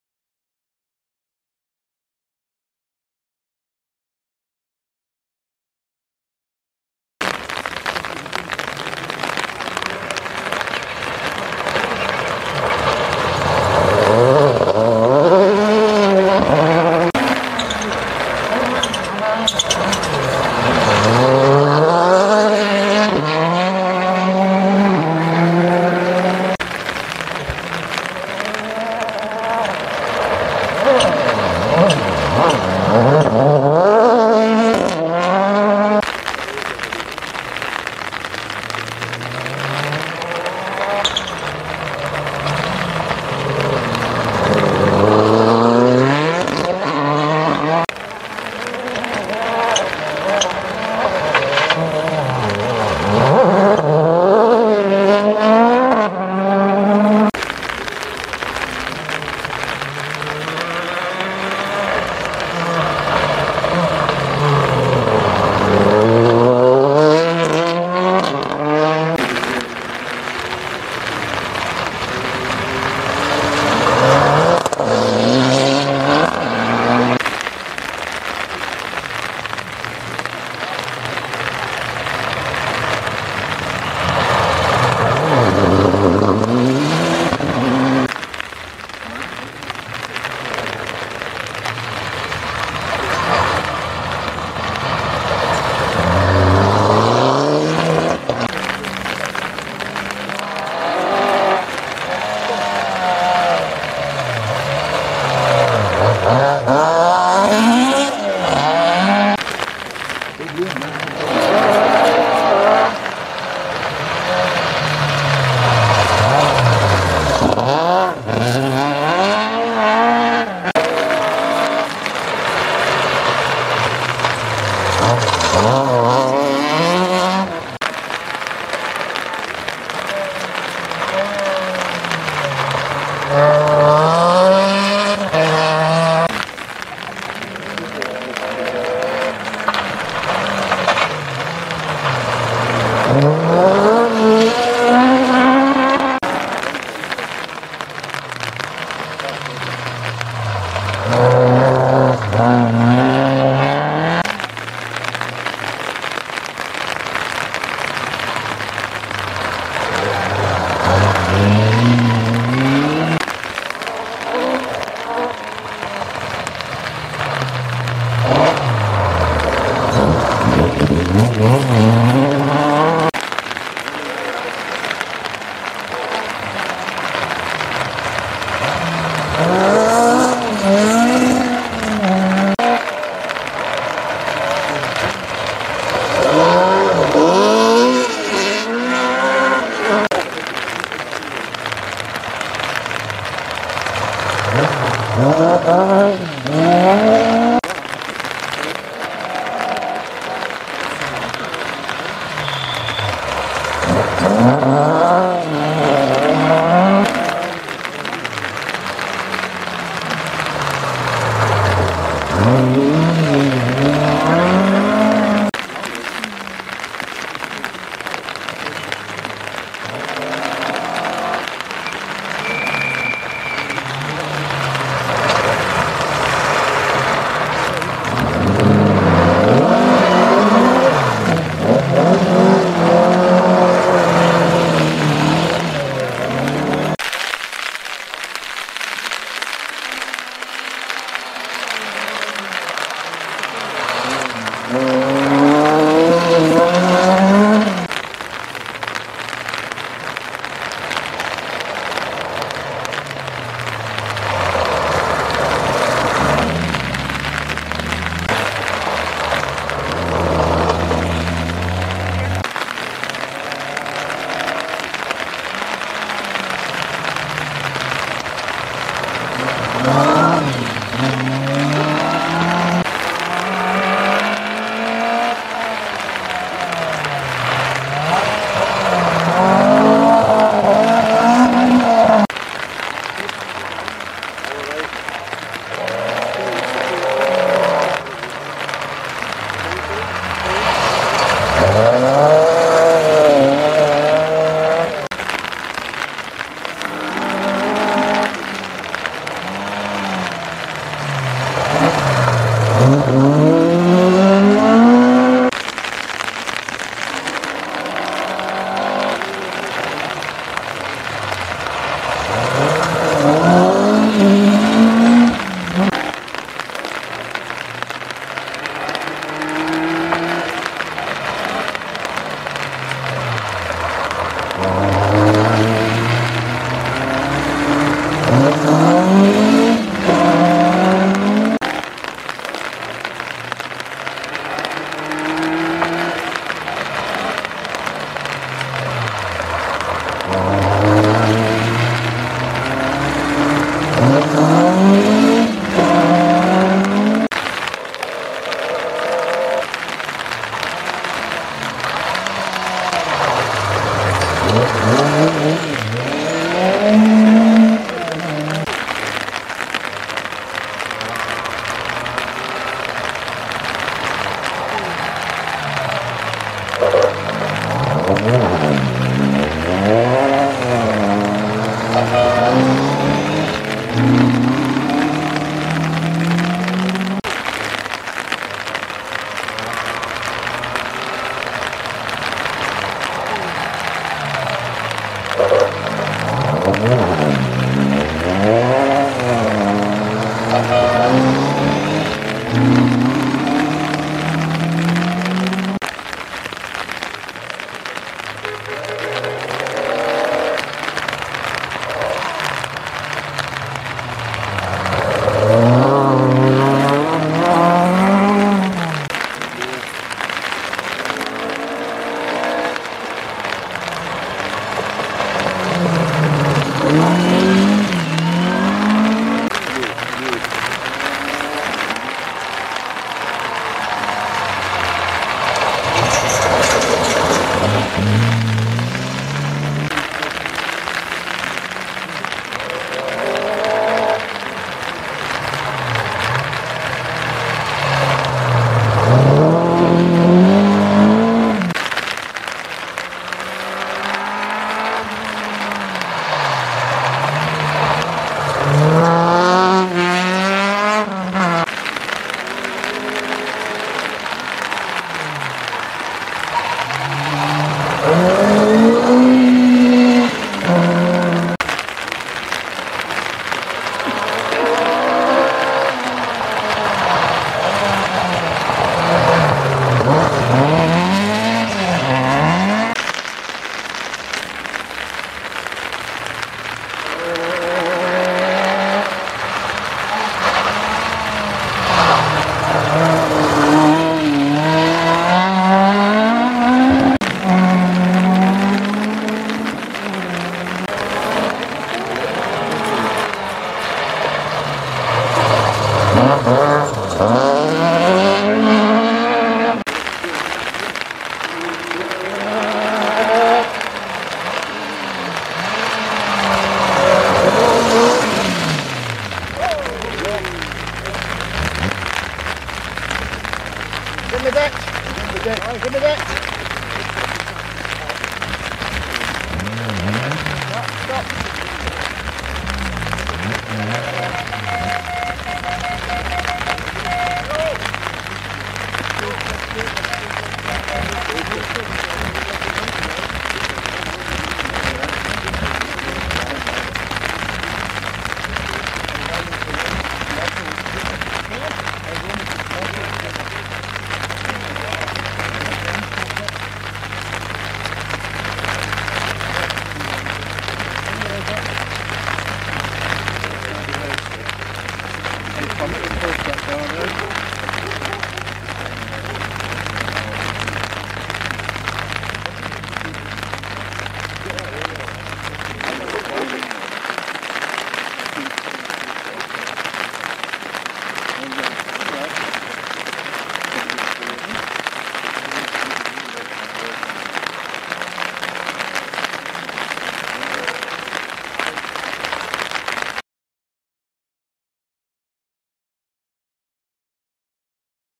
Border Counties Rally 2019
A very wet and very cold Kielder forest.  This is stage 2 pope's hill